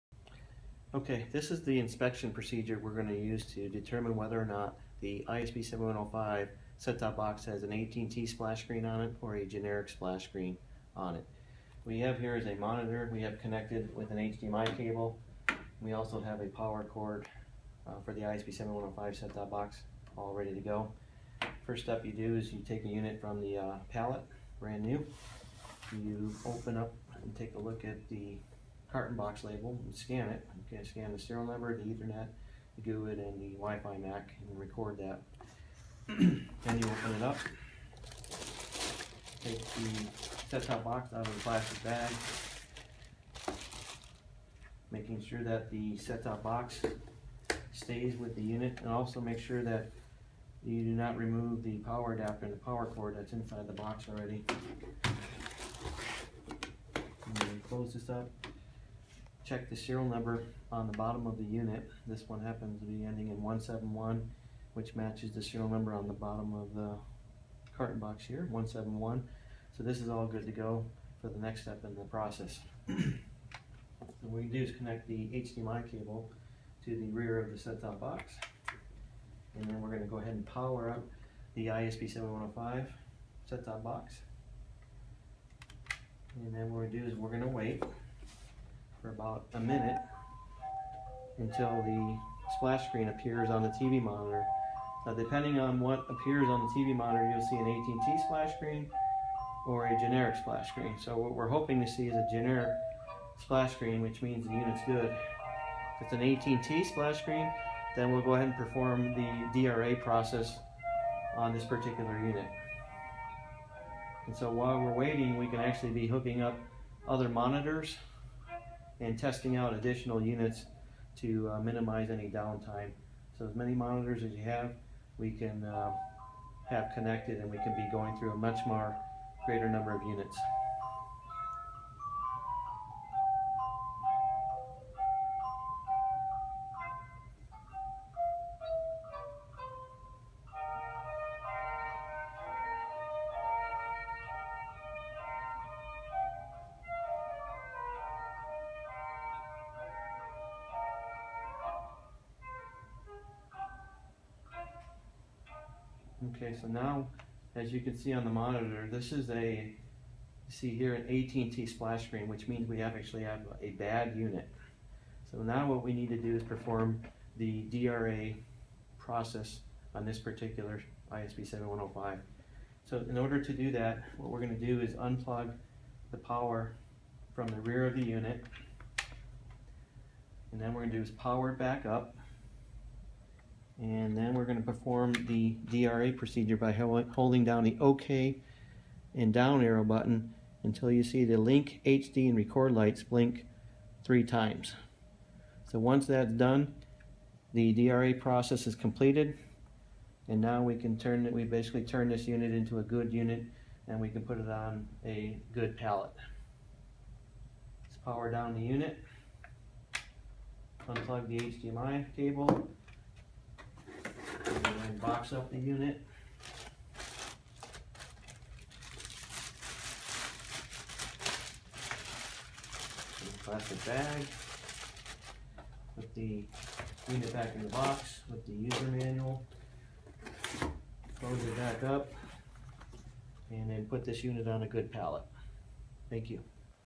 Removing Client from ISB7105
DRA process for clearing the previous client on the Cisco ISB7105 STB and setting the splash screen to factory default.